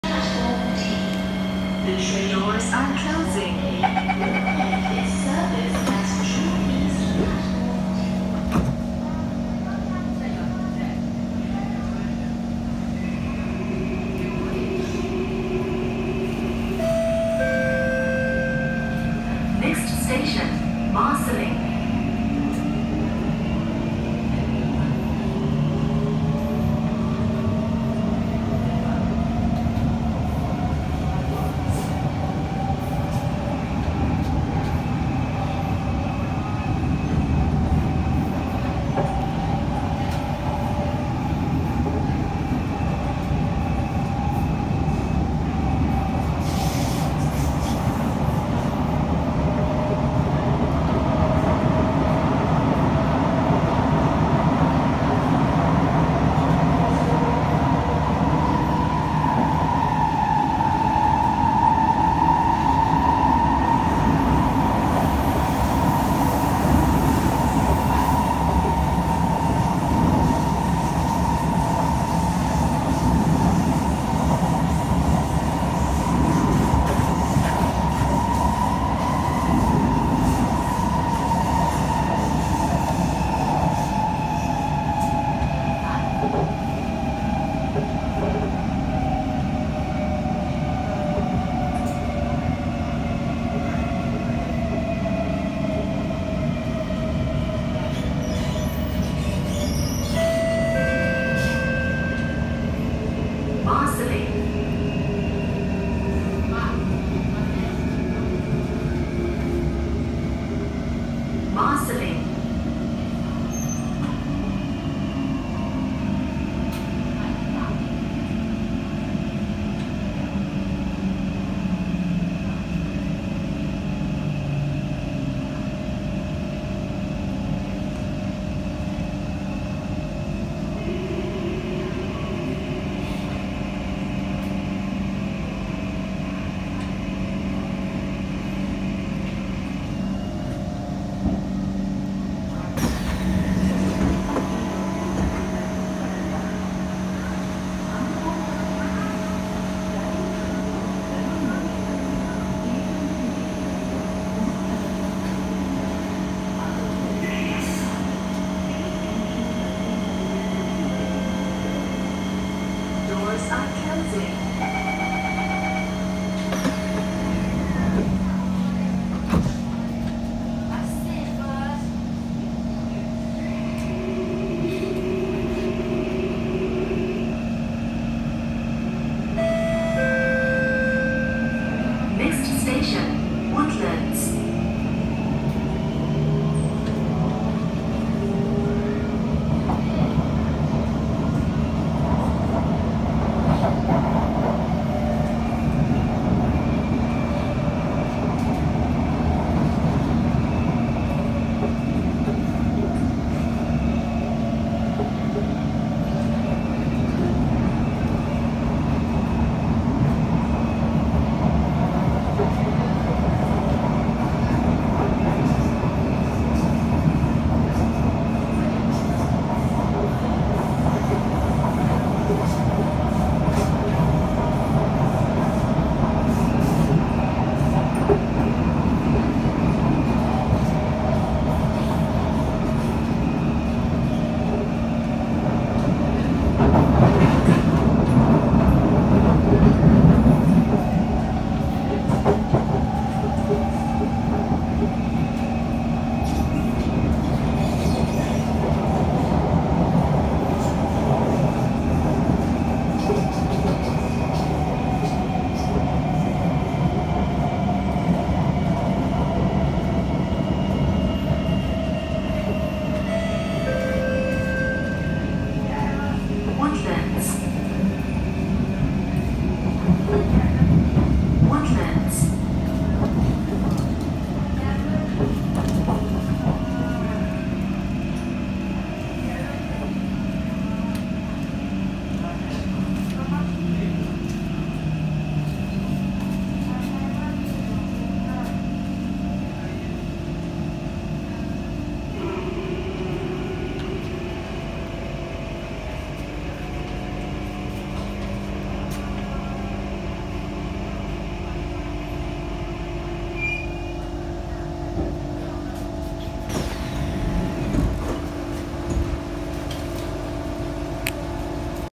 1st Post Circuit breaker full day PMSM NSL - 025/026 on North South Line (Joyride)
SMB3526R Studios films a joyride of a Kawasaki Heavy Industries & Kinki Sharyo C151 (025/026) on North South Line.

With a lifted circuit breaker, I finally get to ride as much non-KSF trains as I can. The 1st PMSM full day NSL was during Circuit Breaker, but speaking of post Circuit Breaker, this was the first PMSM to run full day during post circuit breaker. Alongside, there was  another KHI which was the GTO Chopper that ran full day, total number of 2 off-peak KHIs.

Train Set Number: 025/026
Date of recording: 11 June 2020
Train Running Number: 413 ➡️ 412

Duplicates of this video and channel is not allowed unless permission is given to you.

All videos are owned by SMB3526R Studios unless stated.